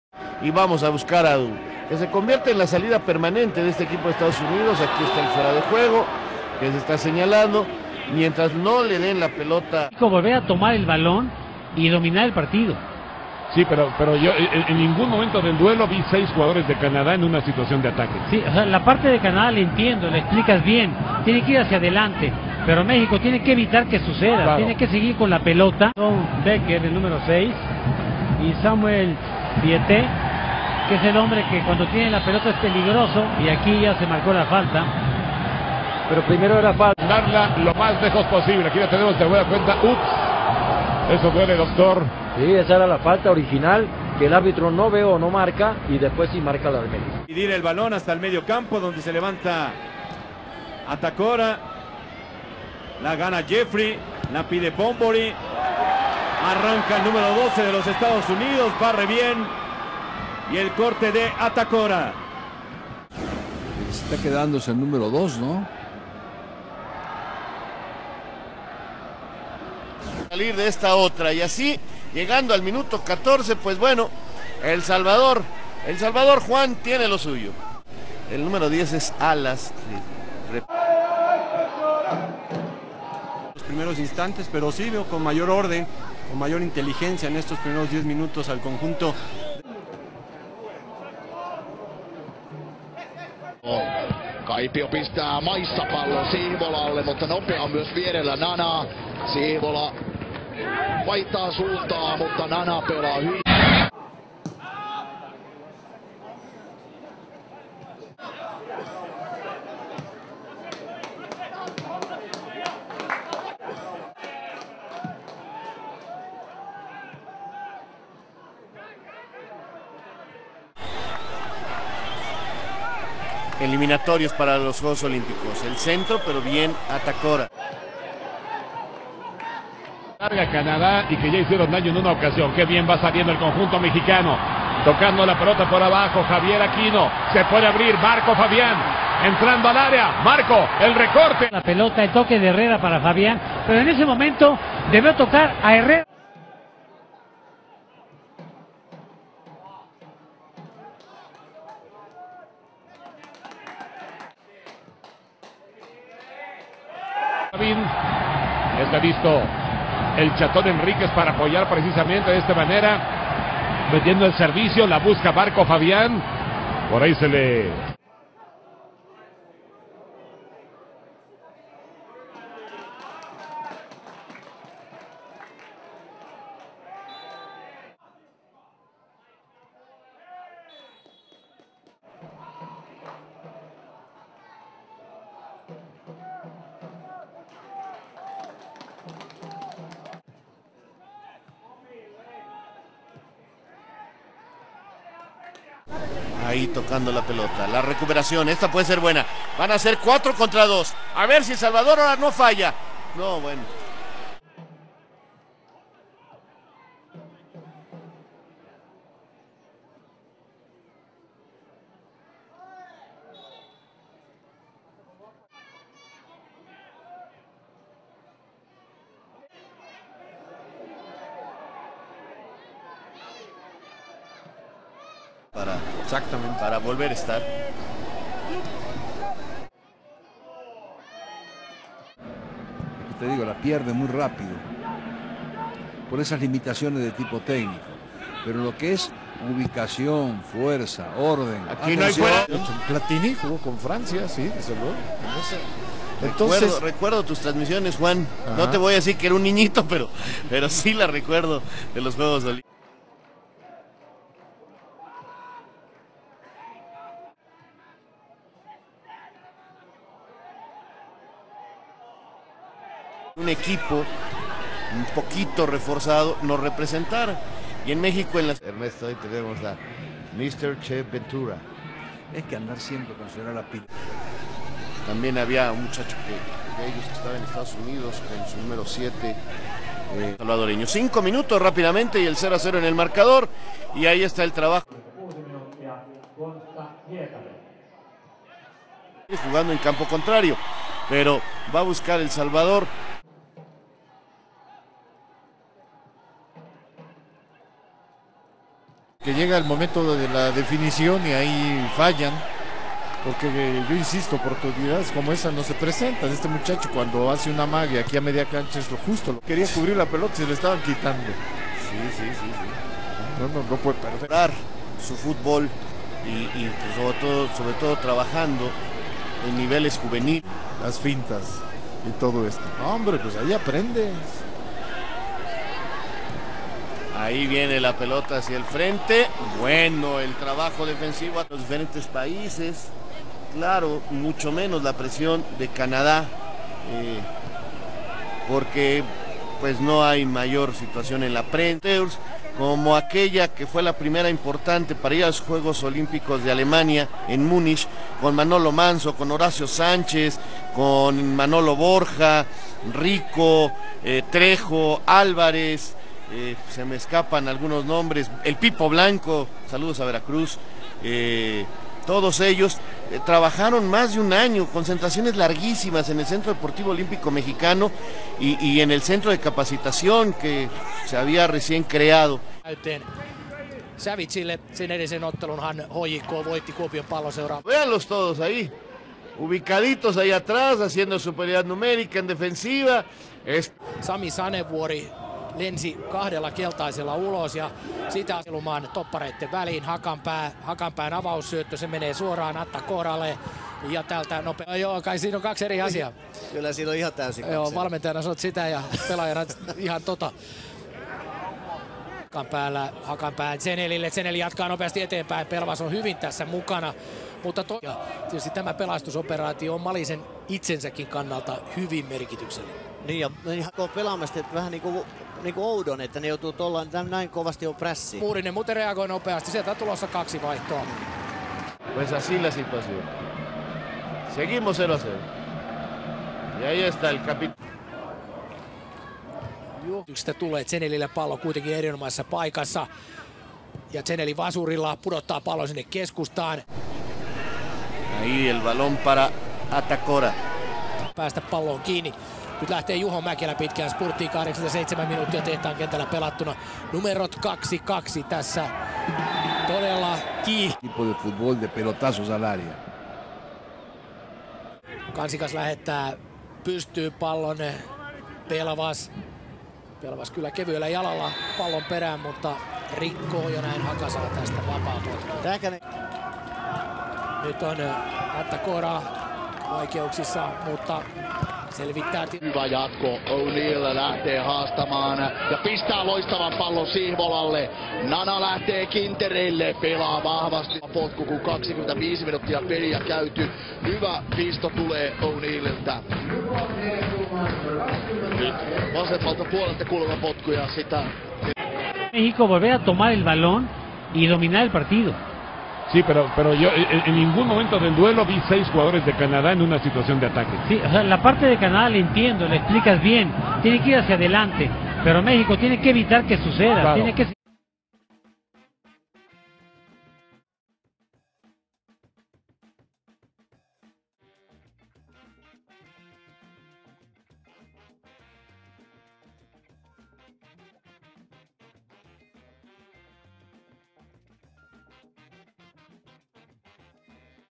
Nana Attakora 2012 Olympic/Haka Highlights
Nana Attakora (born March 27, 1989) is a Canadian soccer player who plays for FC Haka in the Finnish Veikkausliiga.

Attakora has represented Canada at U-16, U-17, U-19, U-20, U-23, and senior team levels. In December 2009, Nana won the Canadian U-20 Players of the Year award for the second consecutive year, after winning the award also in 2008. He made his senior national team debut in a friendly match against Jamaica on January 31, 2010.